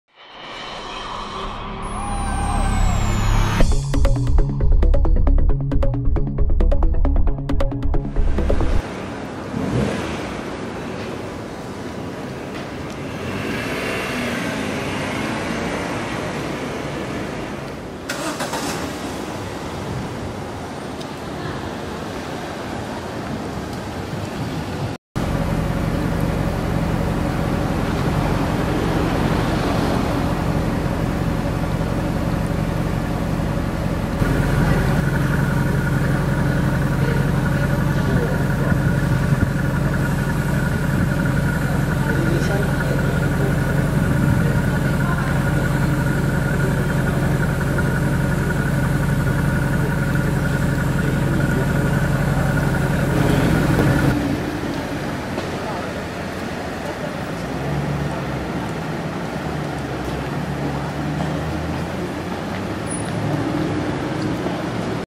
Matte Brown Mercedes SLS AMG Spotted in London.
Simoom Supercars presents this Mercedes SLS AMG spotted in Knightsbridge, London.